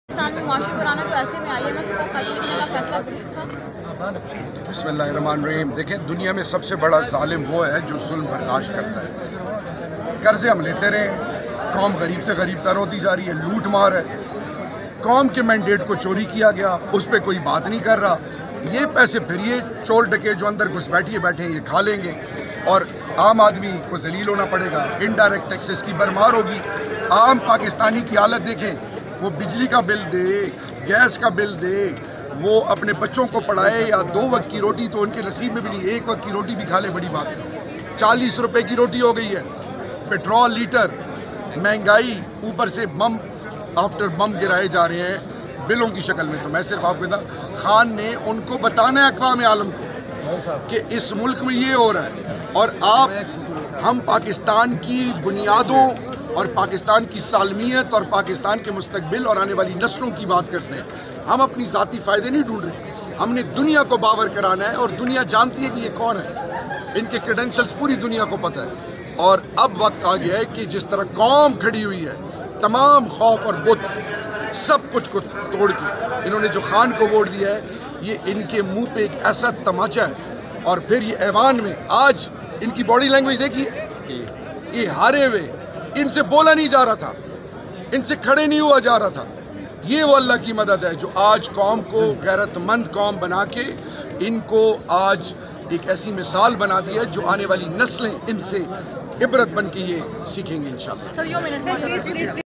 پی ٹی آئی لیڈر شہریار آفریدی کا نیشنل اسمبلی میں پہلا دن | عمران خان کا آئی ایم ایف کو خط لکھنا
پی ٹی آئی لیڈر شہریار آفریدی کا نیشنل اسمبلی میں پہلا دن
 عمران خان کا آئی ایم ایف کو خط لکھنا